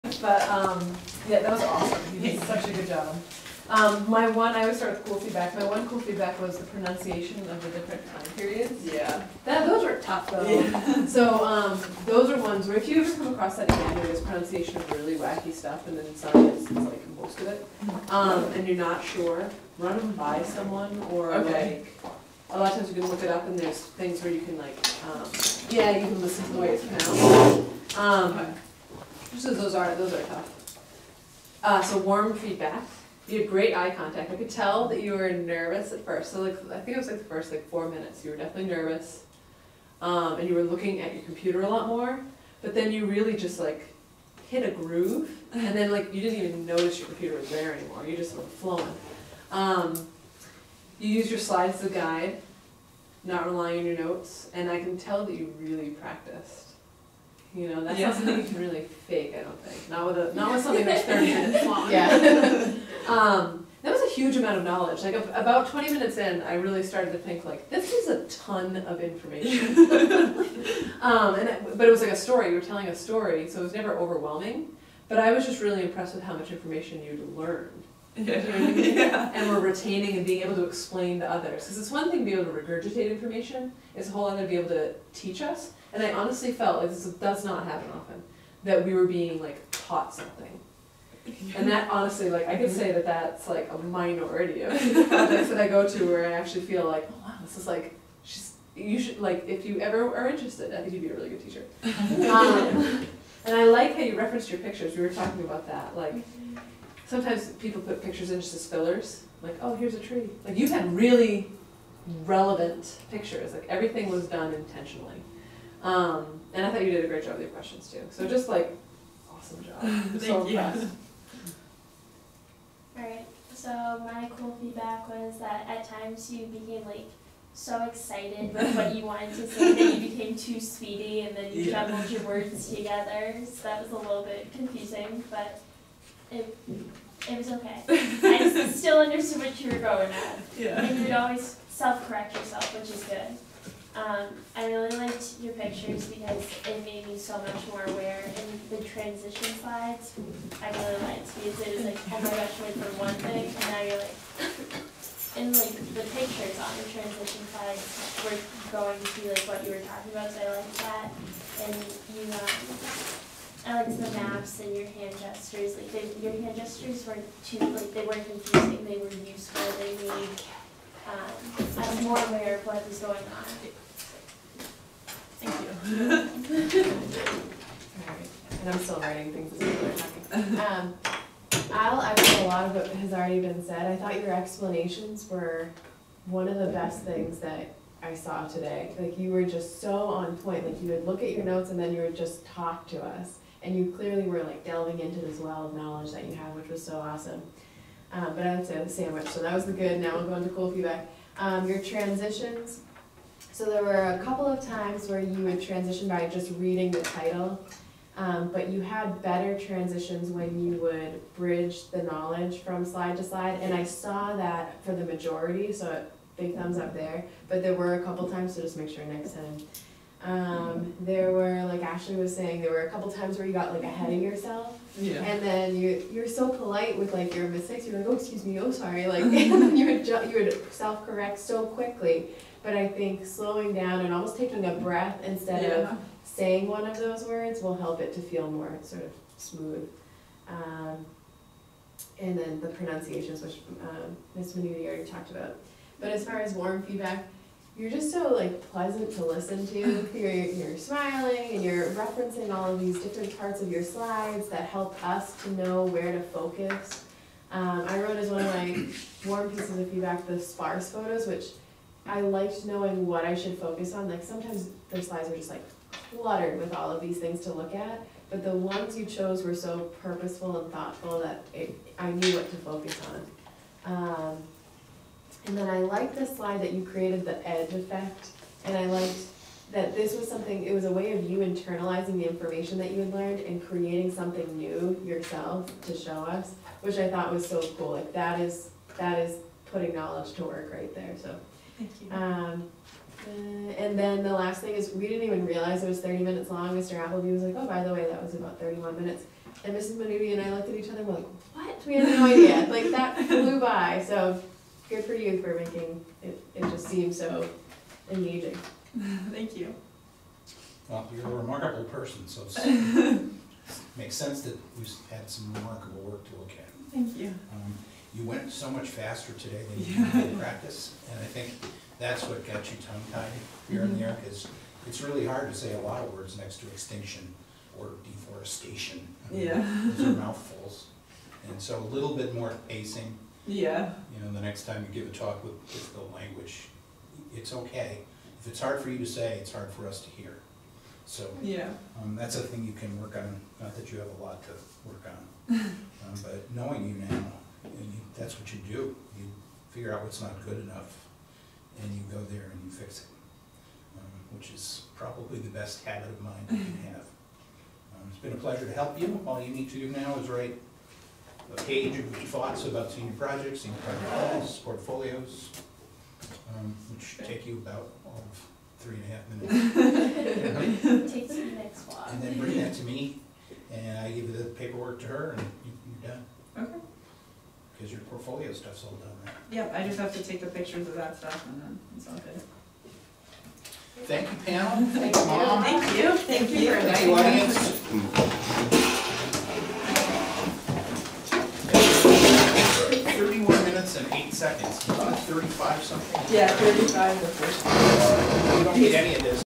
Kendra Silvers SP feedback
The panel gave Kendra feedback after her presentation, which she earned a Distinguished on.